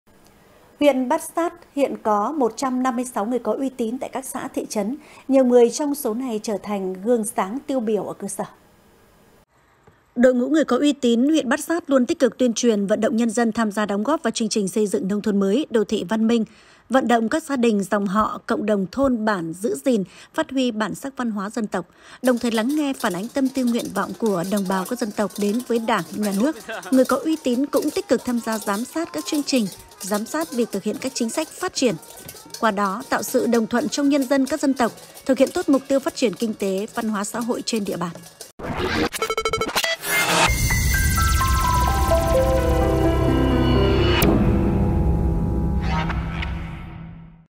Bát Xát có 156 người uy tín | THLC
Laocaitv.vn - Huyện Bát Xát hiện có 156 người uy tín tại các xã, thị trấn, nhiều người trong số này đã trở thành gương sáng tiêu biểu ở cơ sở.
Đài Phát thanh - Truyền hình Lào Cai